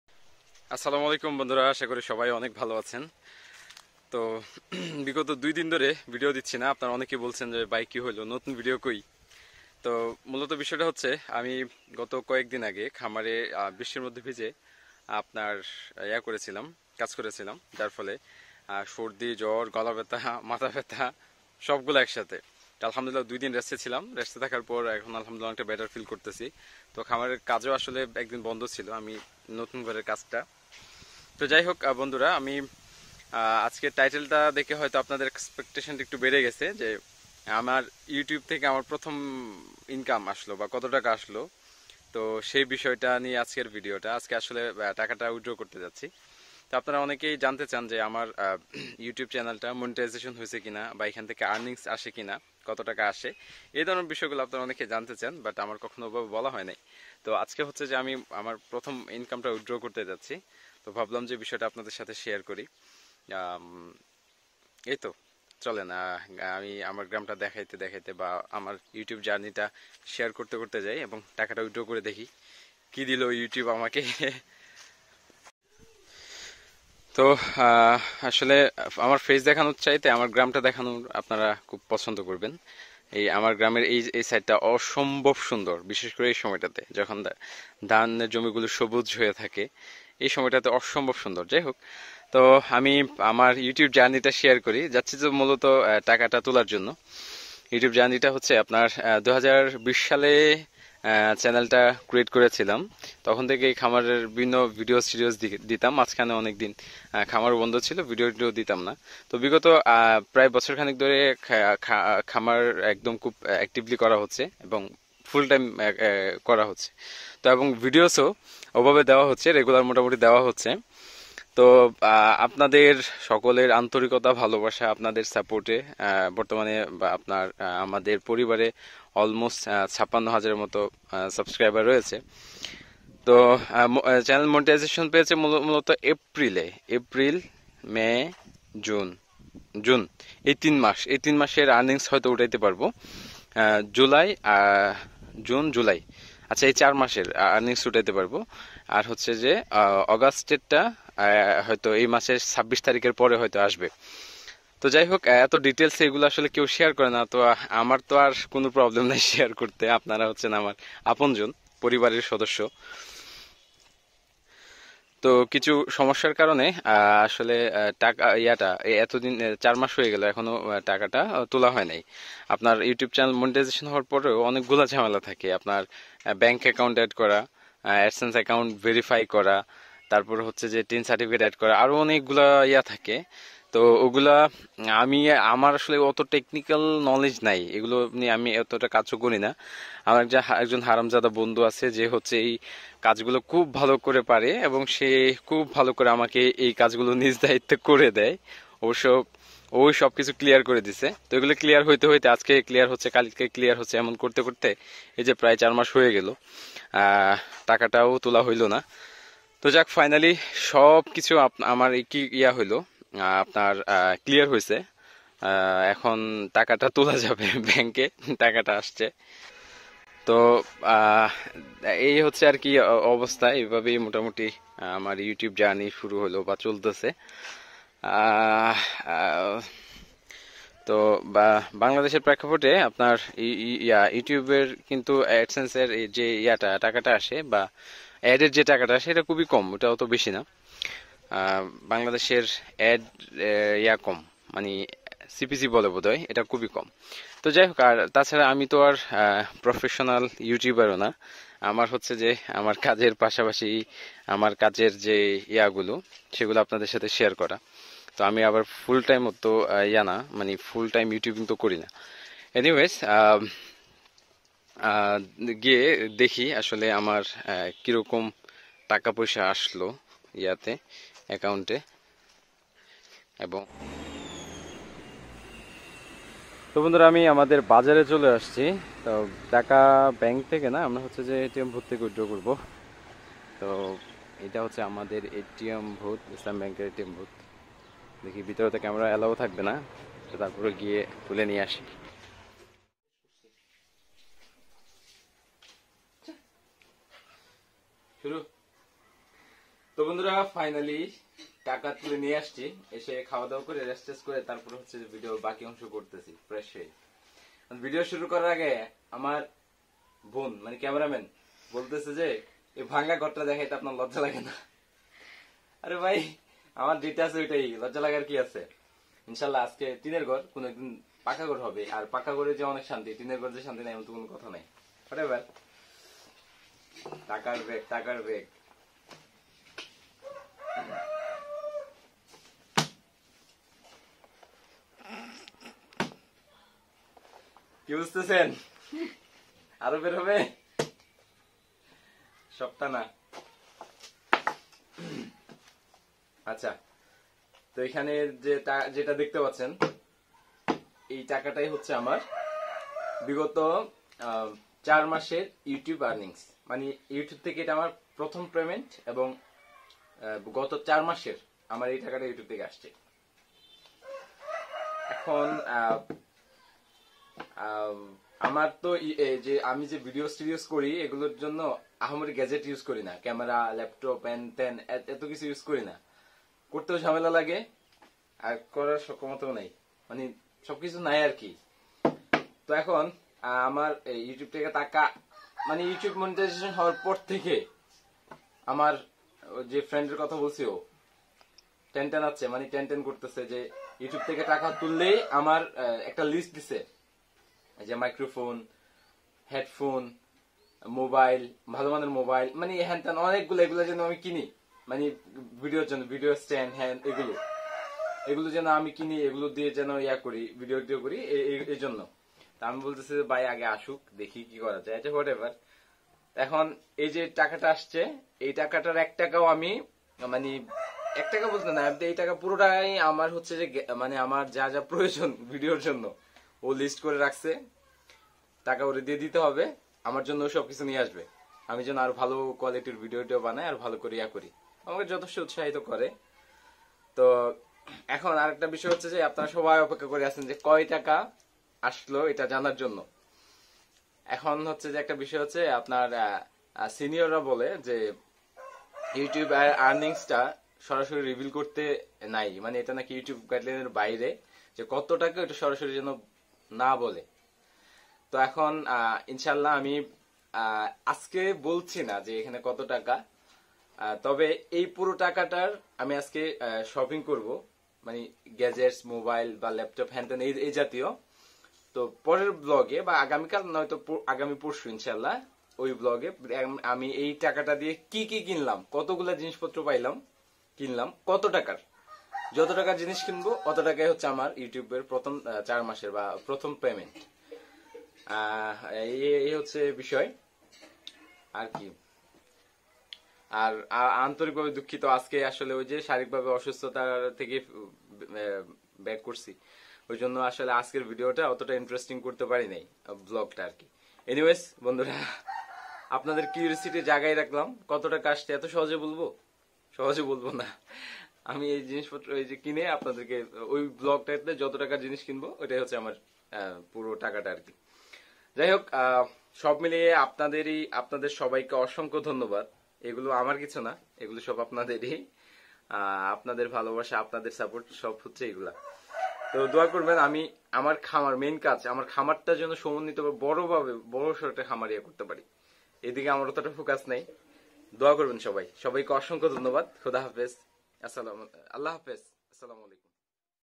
আমাদের ইউটিউব এর প্রথম ইনকাম আজ হাতে পেলাম। BD Agro world
আমাদের ইউটিউব এর প্রথম ইনকাম আজ হাতে পেলাম। কত টাকা পেলাম? টাকাটা কোথায় খরচ করব? BD Agro world